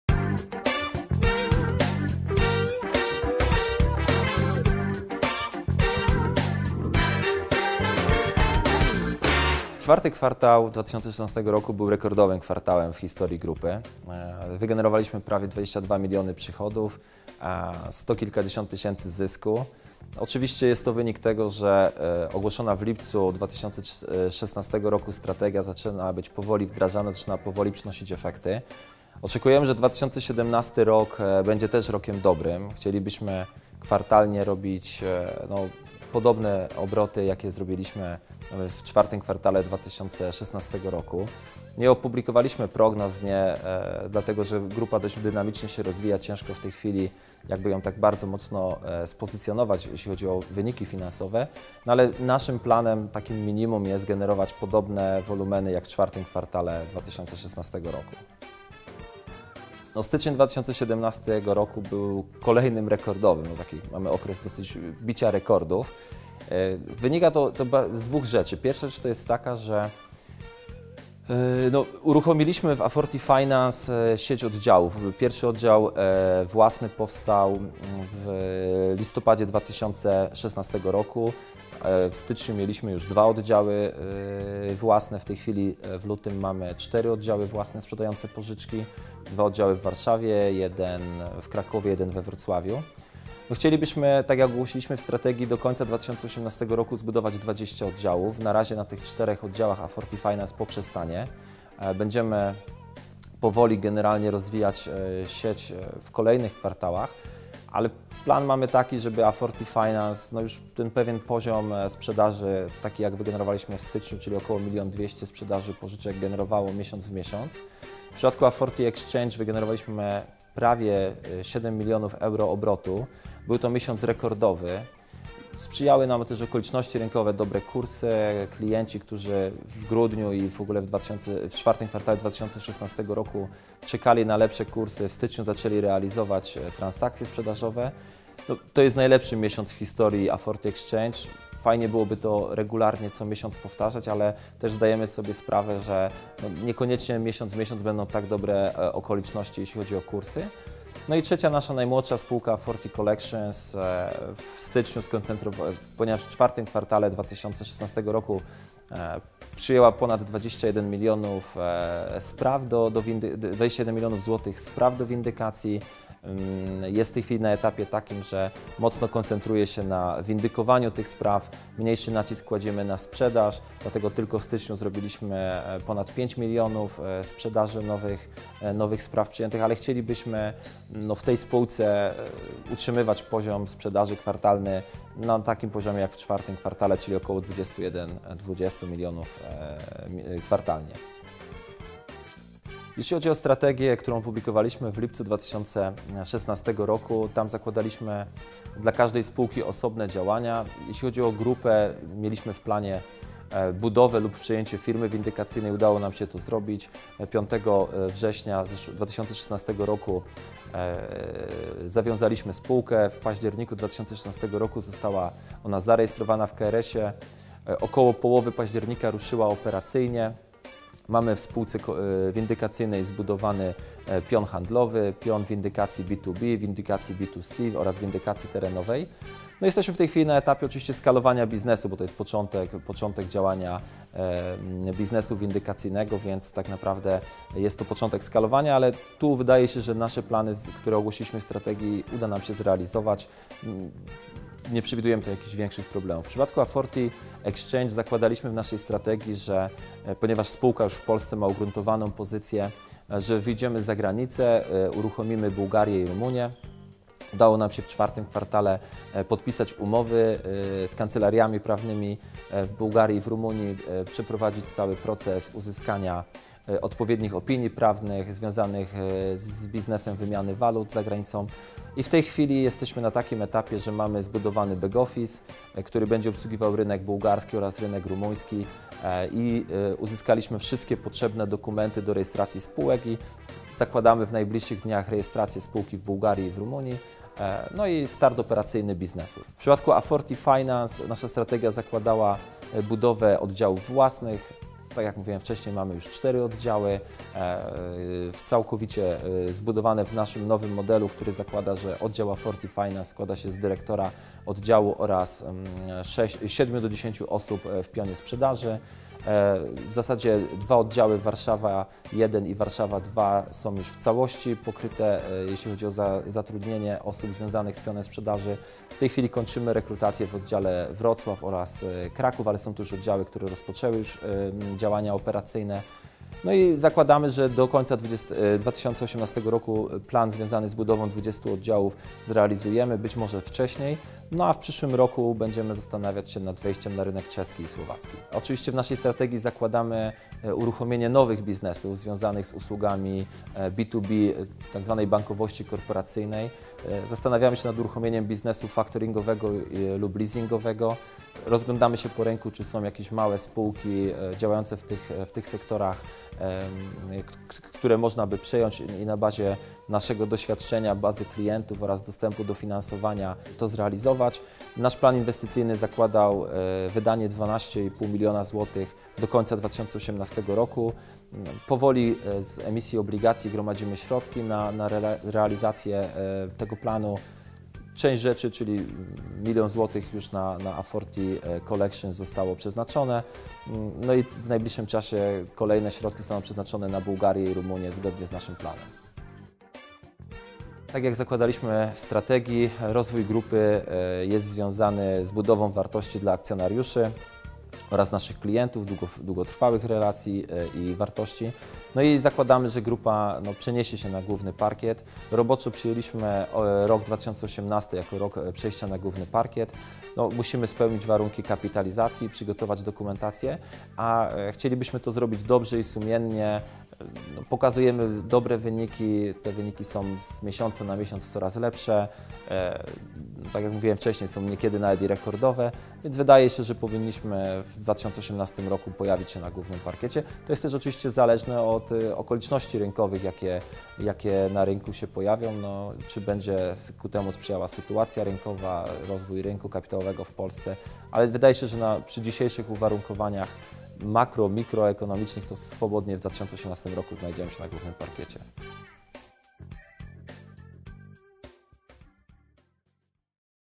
Aforti Holding SA, Klaudiusz Sytek - Prezes Zarządu, #199 ZE SPÓŁEK
O rekordowym dla Grupy IV kwartale 2016 roku, równie dobrym styczniu 2017 oraz kontynuacji dynamicznego rozwoju Klaudiusz Sytek, Prezes Zarządu Aforti Holding SA.